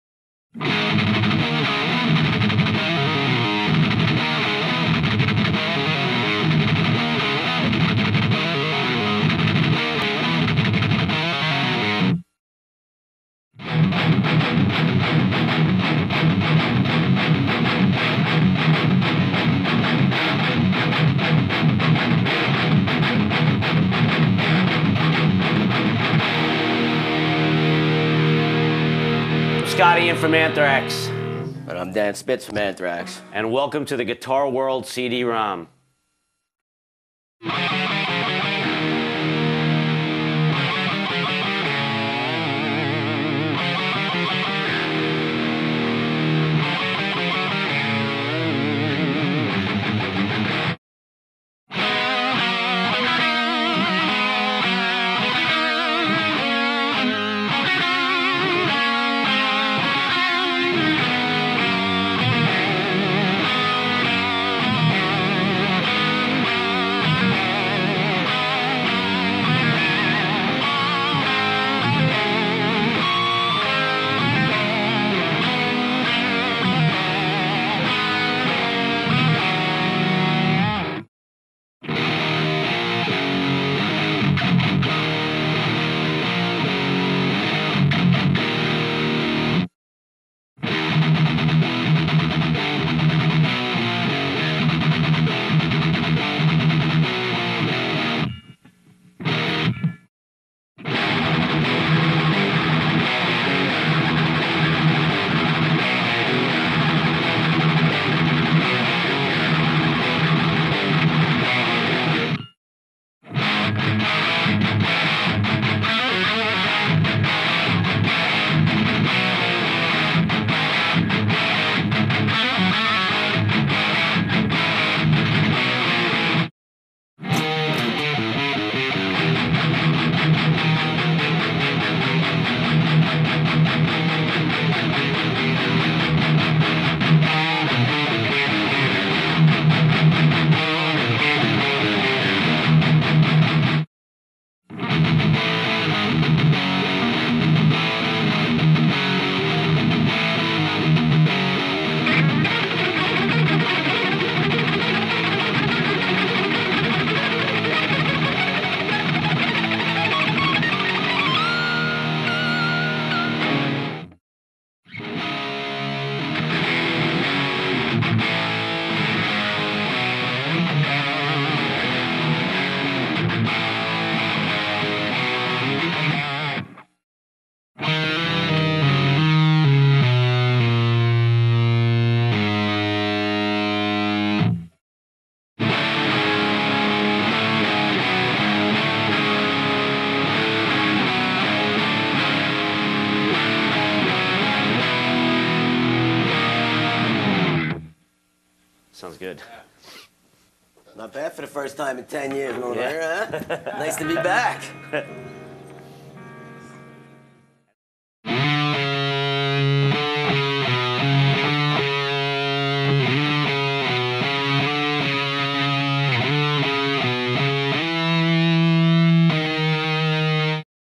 Scott Ian - Anthrax - Guitar Lesson
Scott Ian
Anthrax
Guitar Lesson

Scott Ian (born Scott Ian Rosenfeld; December 31, 1963) is an American musician, author and spoken word storyteller best known as the rhythm guitarist, backing and additional lead vocalist, and the only original remaining member of the thrash metal band Anthrax. He writes the lyrics on all their albums. Ian is the guitarist and a founding member of the crossover thrash band Stormtroopers of Death for which he is also the lyricist. He has hosted The Rock Show on VH1 and has appeared on VH1's I Love the... series, Heavy: The Story of Metal, and SuperGroup. Ian is also the rhythm guitarist for the metal band the Damned Things, and played with experimental band Mr. Bungle for seven reunion shows in 2020.